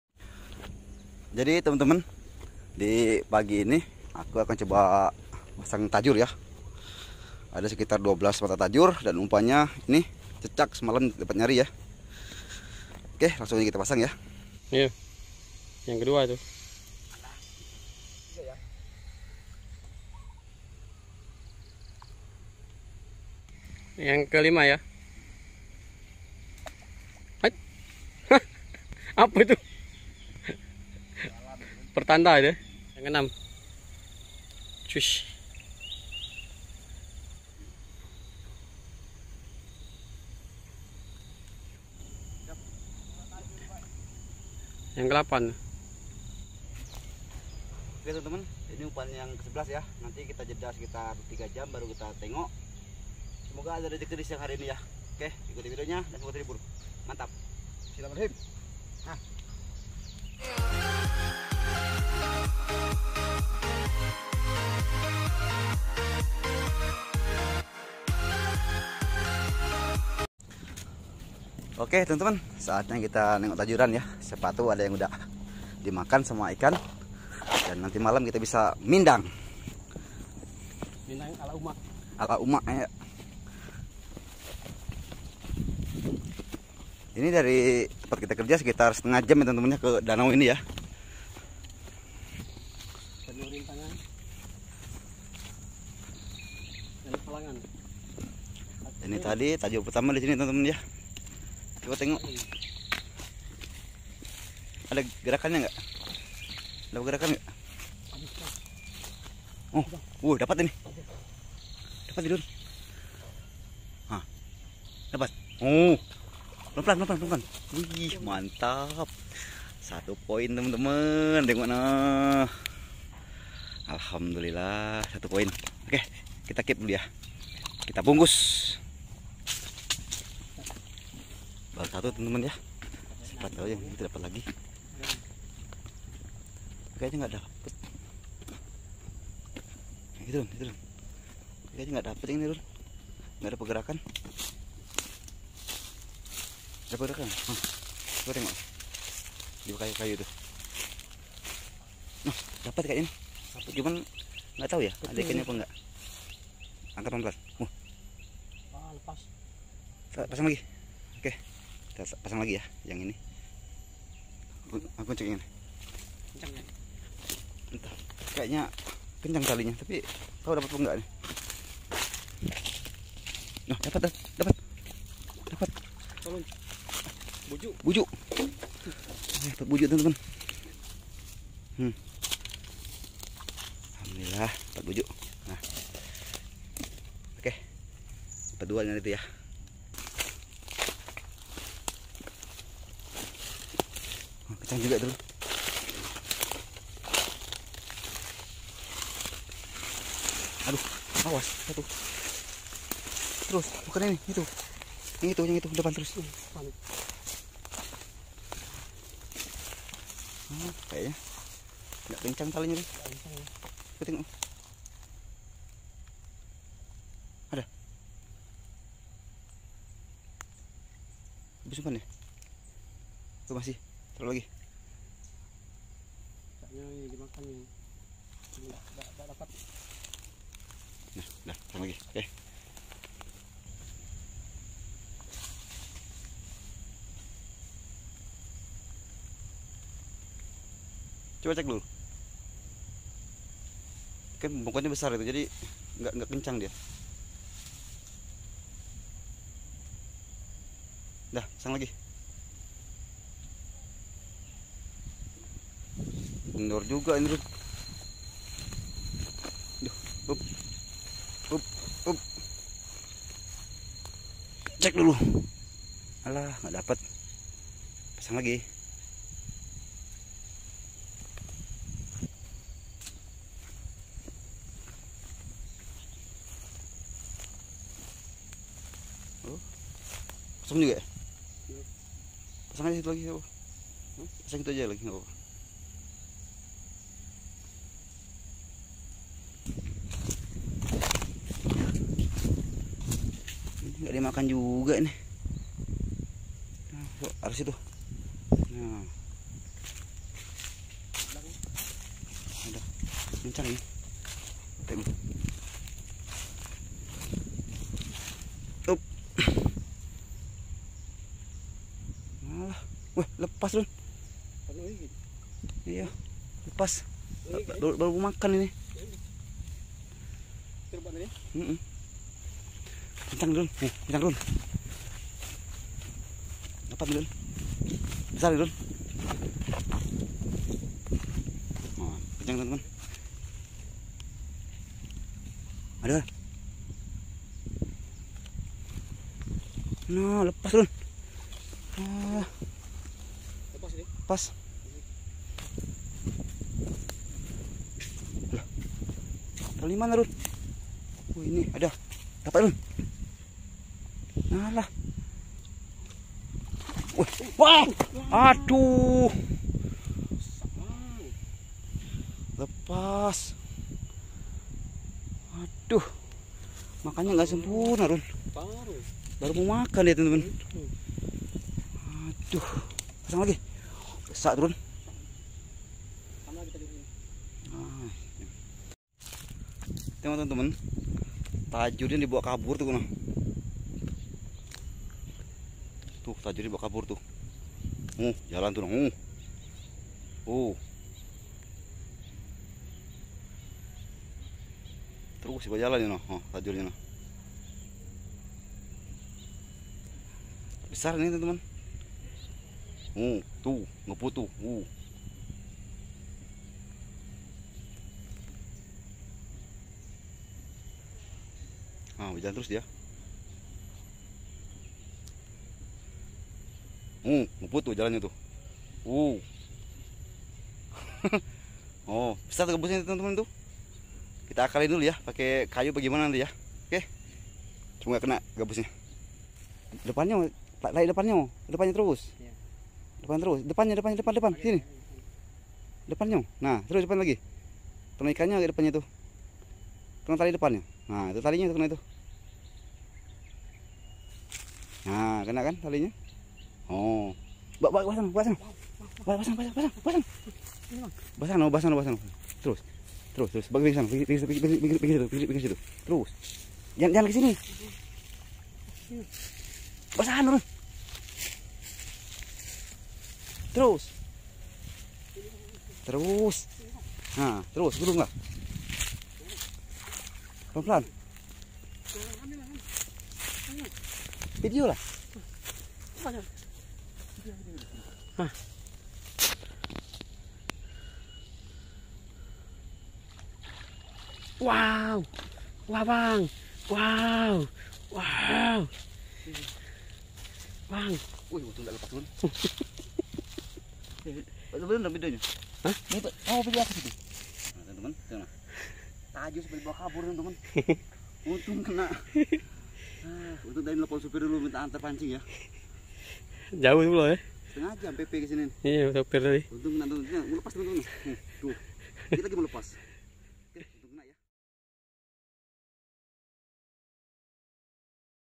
Mancing ikan !! Situasi bikin Panik pemancing ketika umpan di sambar gabus obesitas
Mancing ikan!! situasi bikin Panik ketika umpan di sambar gabus obisitas

mancing gabus umpan cicak
mancing gabus umpan kodok
mancing gabus umpan sepat